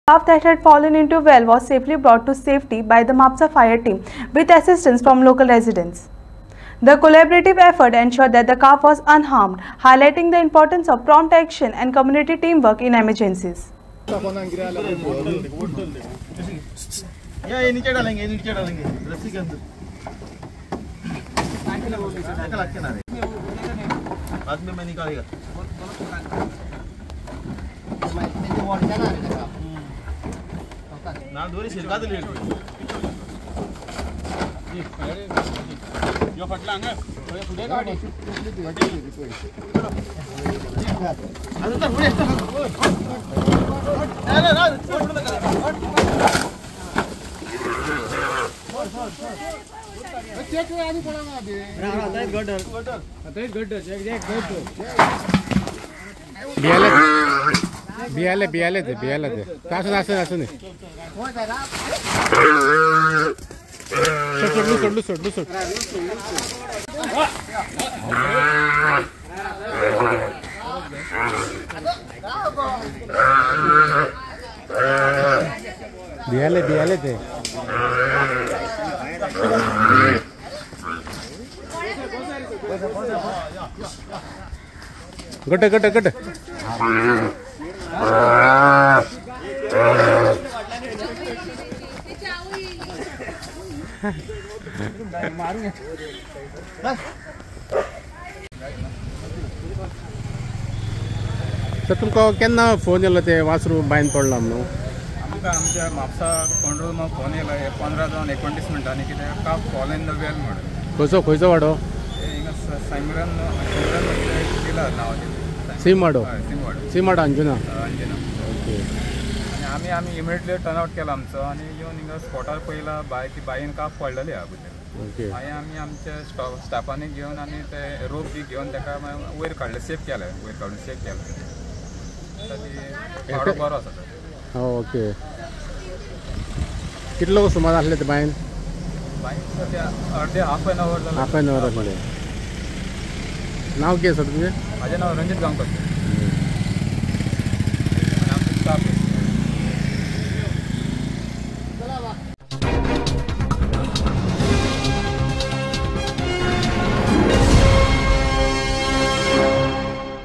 Calf Rescued from Well by Mapusa Fire Team and Local Residents
Quick Response and Community Teamwork Save Calf

In a successful rescue operation in Simer Anjuna, a calf that had fallen into a well was safely brought to safety by the Mapusa fire team, with assistance from local residents. The collaborative effort ensured that the calf was unharmed, highlighting the importance of prompt action and community teamwork in emergencies.

1.
2.
3.
4.
5.